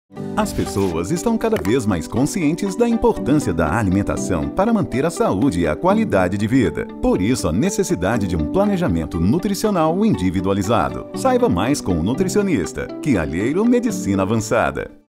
Nutrição | Quialheiro Medicina Avançada
Na Quialheiro Medicina Avançada você encontra profissionais para te ajudar a ter uma alimentação balanceada de qualidade, individualmente ou em grupo, indicando quais alimentos podem ser consumidos para garantir uma alimentação saudável, nutritiva e equilibrada.